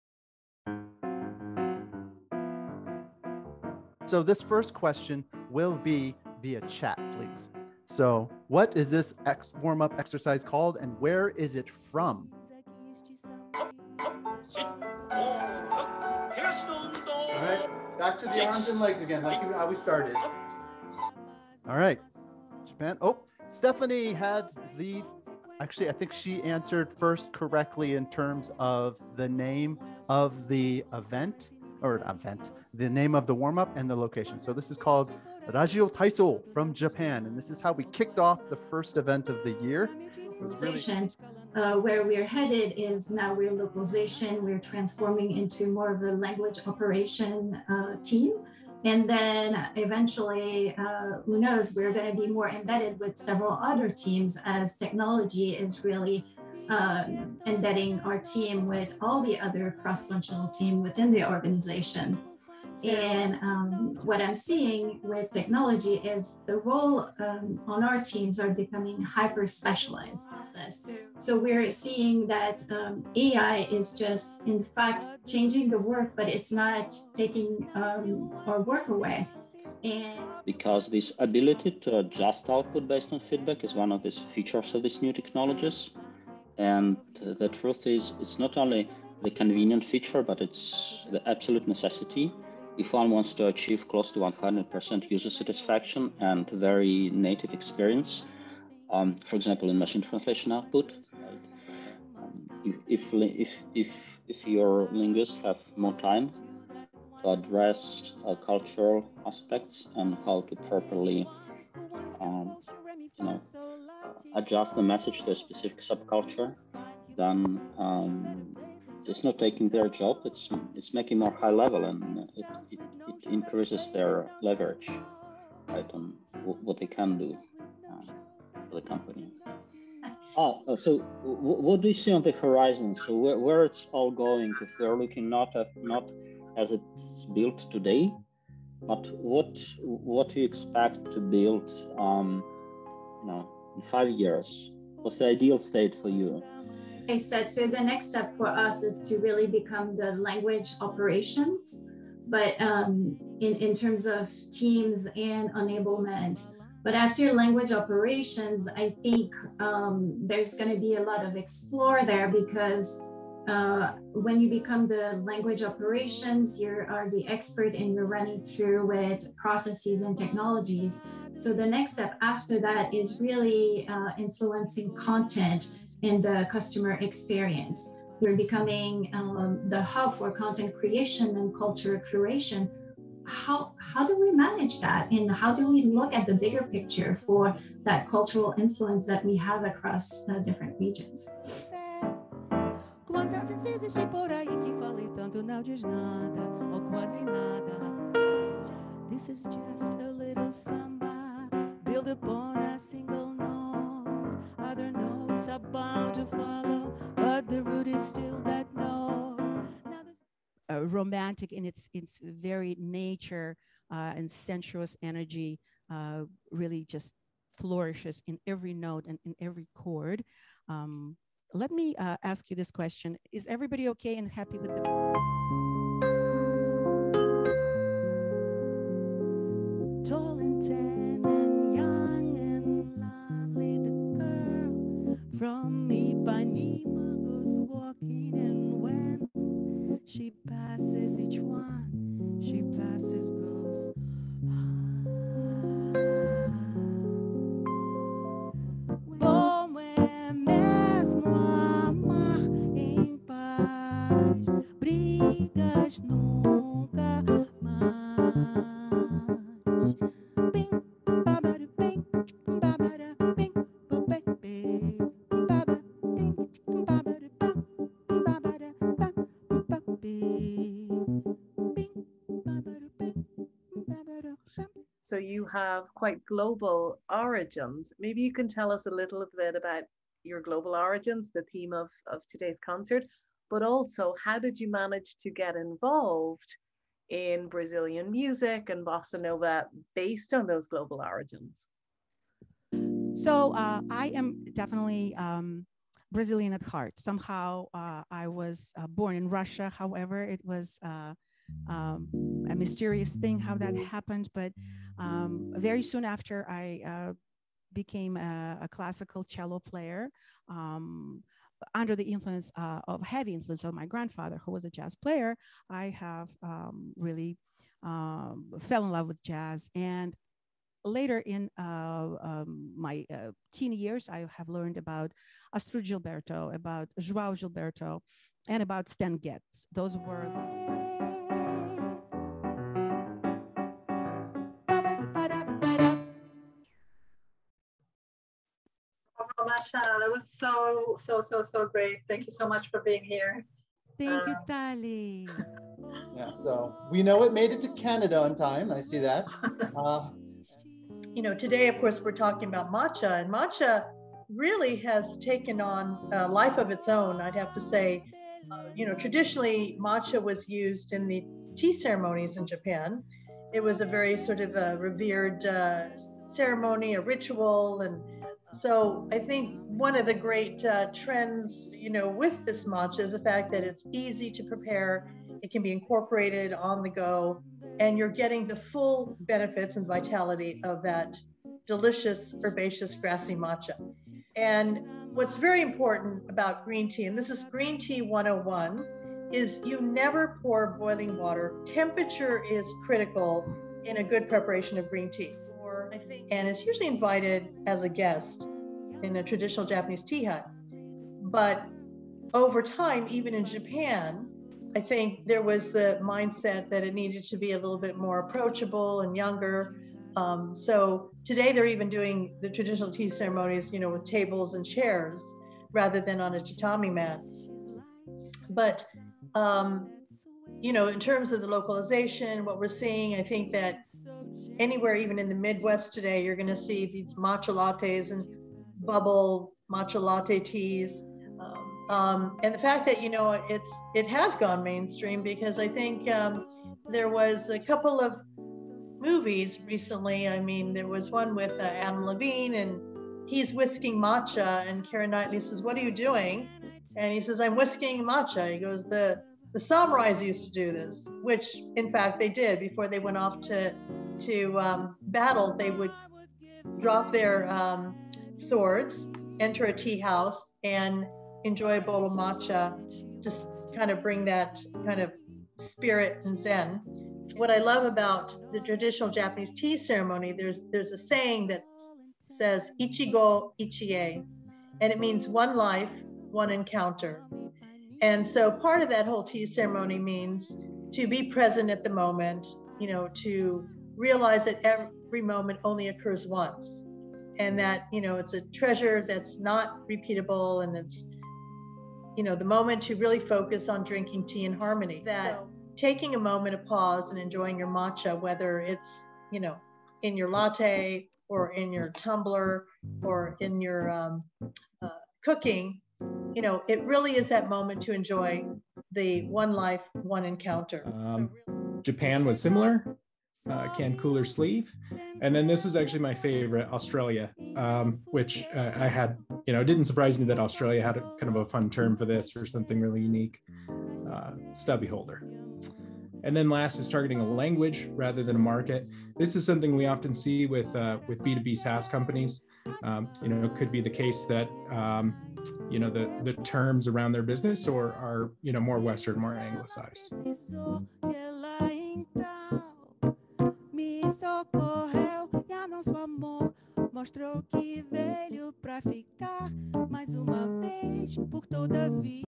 Highlight Reel - December 9 - GlobalSaké 2021 grand finale: “Travel Around the World!"
December 9, 2021. 9am - 12pm PST

GlobalSaké 2021 grand finale: “Travel Around the World!"

After a full year of monthly interactive meetups at The ParlamINT program, exploring solutions to core international challenges with familiar and new faces across the globe, get ready to party at our grand finale wrap-up virtual event, featuring a Brazilian live concert, Matcha Tasting, Open Mic networking and content sessions.